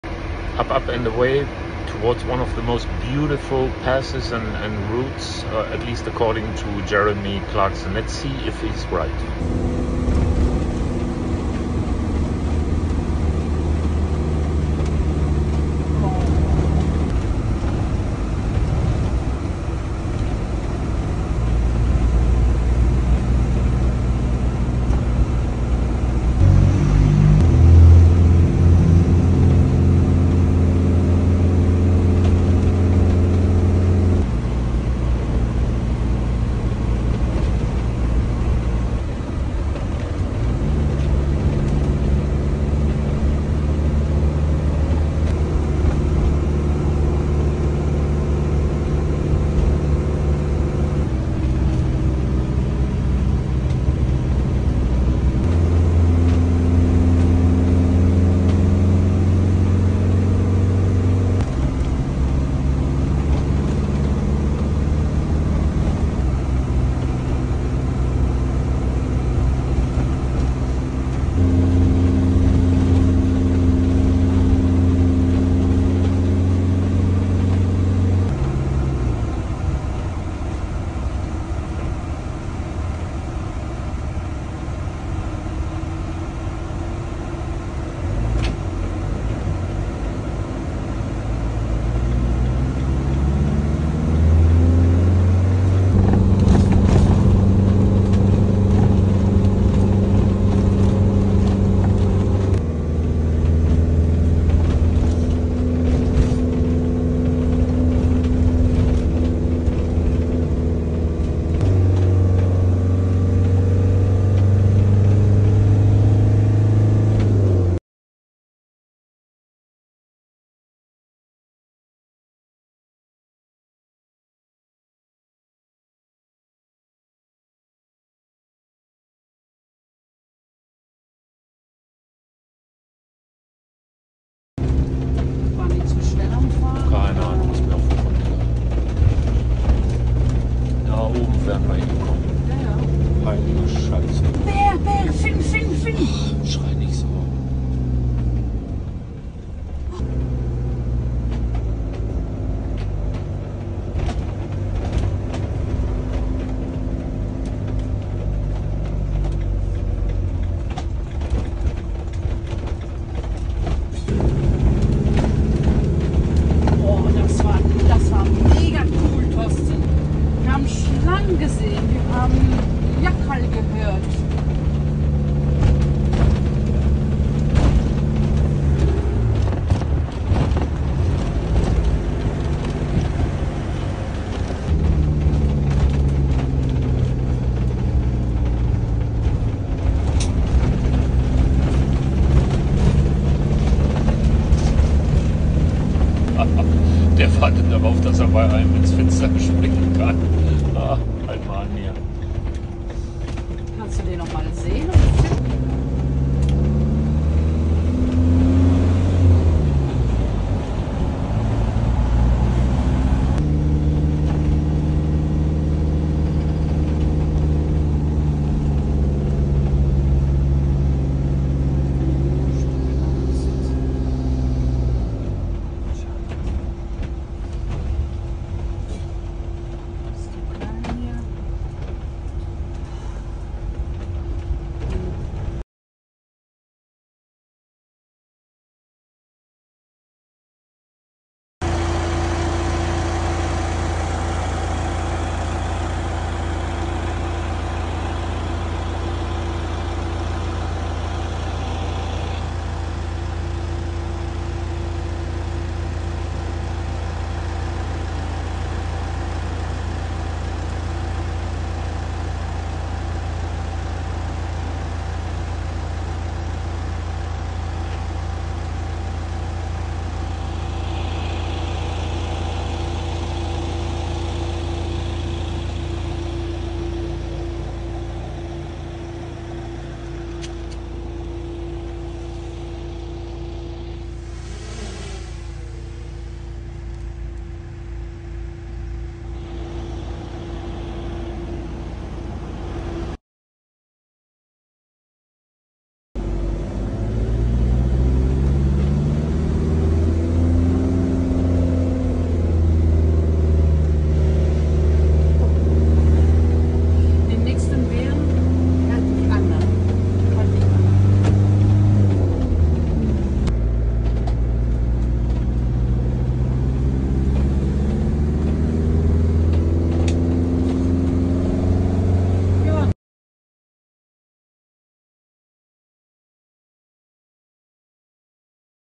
The Transfaragasan: Is it the most beautiful route in the world?
28 viadukts, 833 bridges, 4 tunnels and an endless amount of curves, turns and serpentines. Highest point? The Bâlea Lake at 2042 meters.